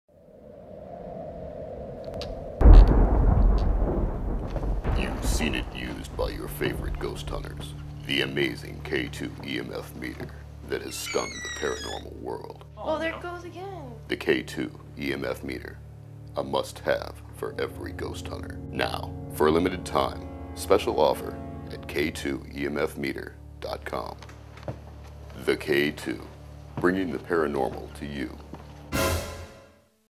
TV/Web Commercial, Client: Ghost Augustine, Saint Augustine, Florida
This is a Television/Web commercial put together for a Saint Augustine, FL Ghost Tour company, Ghost Augustine. Shot in the very spooky Kenwood Inn, this 30 second commercial's goal was to market a ghost detection devise much like the ones seen on "Ghost Hunters".